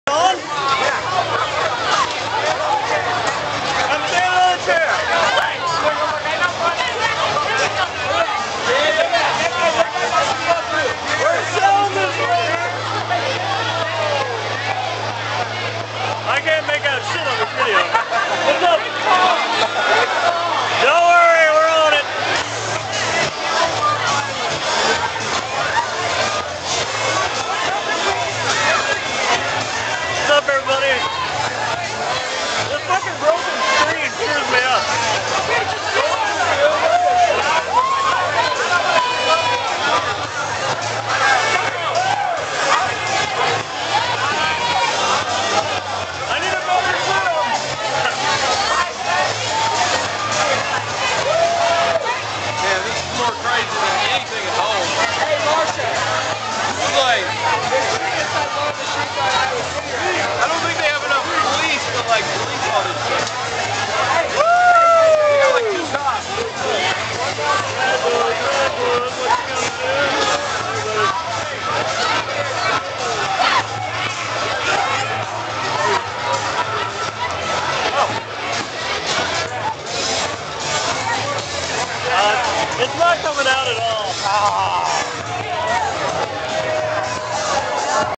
Warrensburg UCM (CMSU) 2007 Homecoming Chaos! *Working Link*
This video was taken from the Warehouse Bar on Pine St. As you can see Pine St was completely packed with people. This video was taken about 30 minutes before the chaos erupted & the COPS broke out the pepper spray. Things started to get rowdy & people were running & screaming. Good times!